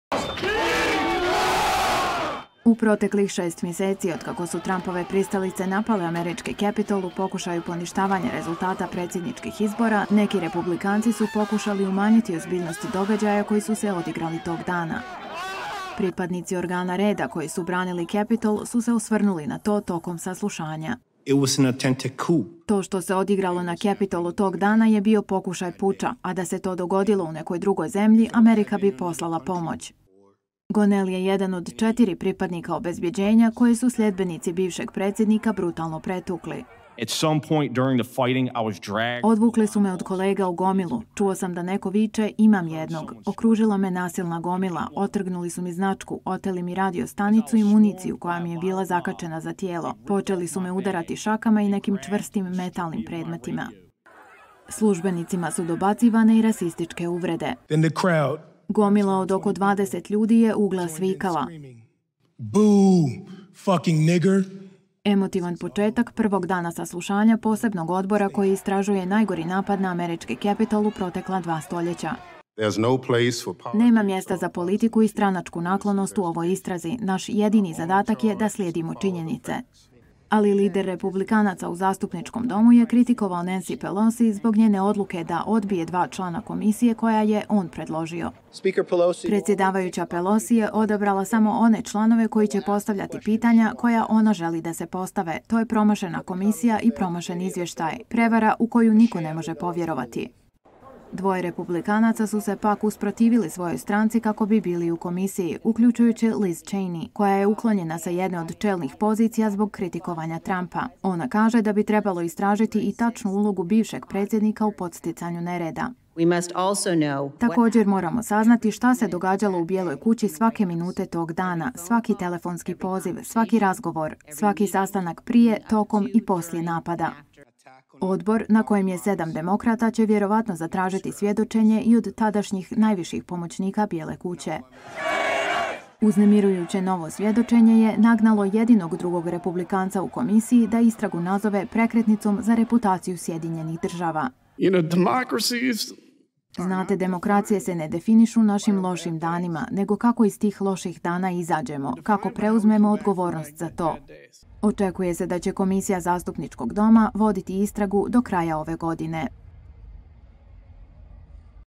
Policijski službenici opisali napad na Capitol američkim zakonodavcima
Američki zakonodavci su čuli emotivna svjedočenja četiri pripadnika organa reda tokom zasjedanja specijalnog odbora Zastupničkog doma koji istražuje napad na Kongres od 6. januara.